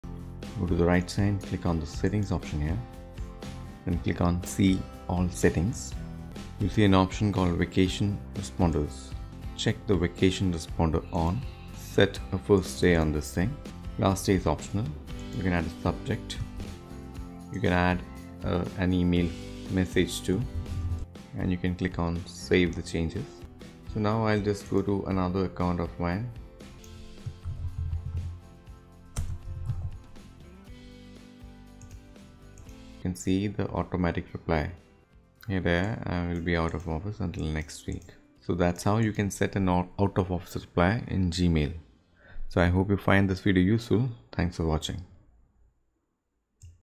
Setting Up Out of Office in Gmail | Vacation Responder
In this video, you'll see how to set up an out of office reply in Gmail for auto reply when you are unavailable for mail replies.

Setting up vacation responders for gmail.